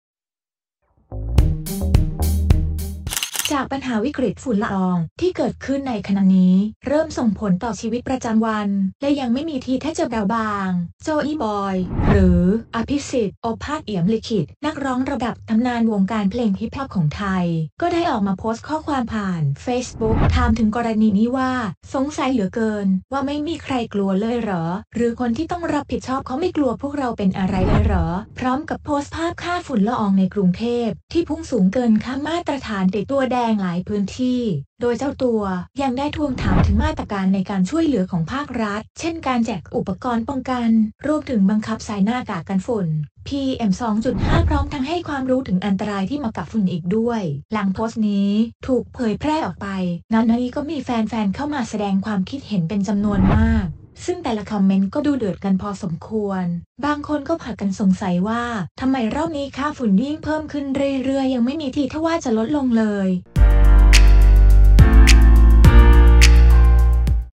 โจอี้ บอย ตั้งคำถาม ปม ฝุ่น PM2.5 จี้ คนที่ต้องรับผิดชอบ ห่วงกันบ้างไหม?| Springnews | 20ม.ค.63
โจอี้ บอย นักร้องแร็ปเปอร์ชื่อดังของไทย ออกมาโพสต์ข้อความผ่านเฟซบุ๊กถามถึงปมนี้ว่า "สงสัยเหลือเกิน ว่าไม่มีใครกลัวเลยเหรอ ? หรือคนที่ต้องรับผิดชอบเขาไม่กลัวพวกเราเป็นอะไรเลยเหรอ ?" พร้อมกับโพสต์ภาพค่าฝุ่นละอองในกรุงเทพฯ ที่พุ่งสูงเกินค่ามาตรฐานติดตัวแดงหลายพื้น